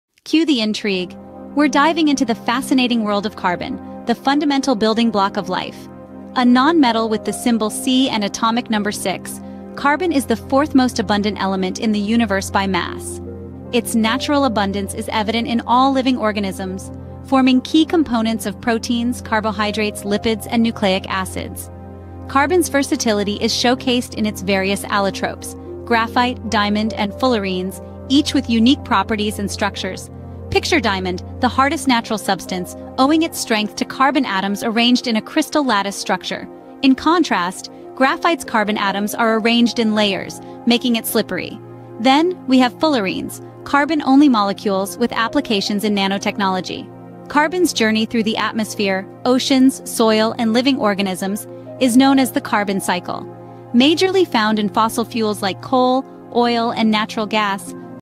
10 Fascinating Facts About Carbon Shorts
Carbon is a chemical element with the symbol "C" and atomic number 6. It is a non-metal and is the fourth-most abundant element in the universe by mass. Carbon is the fundamental building block of life, and its ability to form diverse compounds is the basis for the complexity and variety of organic molecules.

Here are some key points about carbon:

Natural Abundance: Carbon is found in all living organisms and is a key component of organic compounds, including proteins, carbohydrates, lipids, and nucleic acids.

Allotropes: Carbon exists in various allotropes, including graphite, diamond, and fullerenes. Each allotrope has unique properties and structures.

Diamond: In its diamond form, carbon atoms are arranged in a crystal lattice structure, making diamond the hardest natural substance.

Graphite: In graphite, carbon atoms are arranged in layers, allowing for the slippery and lubricating properties of graphite.

Fullerenes: Fullerenes are molecules composed entirely of carbon, such as buckyballs and carbon nanotubes, which have unique properties with applications in nanotechnology.

Carbon Cycle: The carbon cycle is a natural process through which carbon is exchanged between the atmosphere, oceans, soil, and living organisms.

Fossil Fuels: Carbon is a major component of fossil fuels, including coal, oil, and natural gas. The combustion of these fuels releases carbon dioxide into the atmosphere, contributing to climate change.

Carbon Compounds: Carbon forms covalent bonds with other elements, creating a vast array of organic compounds essential for life. Carbon's versatility allows for the diversity of molecules found in living organisms.

Carbon Dating: Carbon-14, a radioactive isotope of carbon, is used in carbon dating to determine the age of organic materials.

Carbon Footprint: The term "carbon footprint" refers to the total amount of greenhouse gases, primarily carbon dioxide, that an individual, organization, or activity releases into the atmosphere, contributing to climate change.

Carbon's importance extends across various scientific disciplines, from biology and chemistry to materials science and environmental science. Understanding carbon and its compounds is crucial for comprehending the foundations of life and the impact of human activities on the environment.